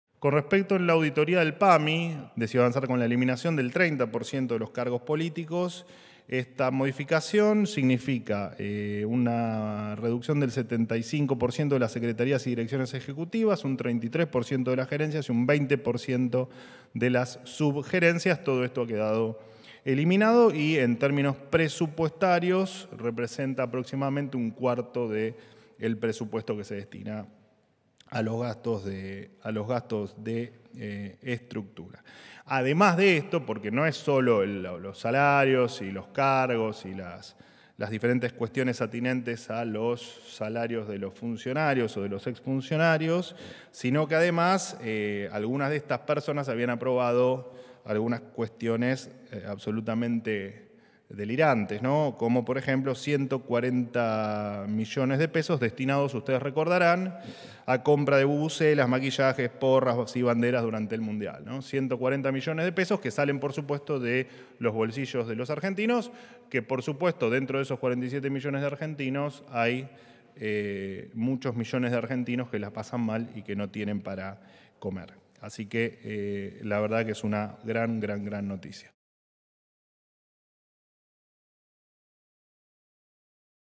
AUDITORÍA EN PAMI REDUCEN 30% DE CARGOS
La Dirección Ejecutiva del PAMI avanzó en la reducción del 30% de los cargos políticos y señaló que esta “limpieza del empleo militante” representa “un cuarto del presupuesto que se destina a los gastos de su estructura”
ENTREVISTAS:
MANUEL ADORNI - VOCERO PRESIDENCIAL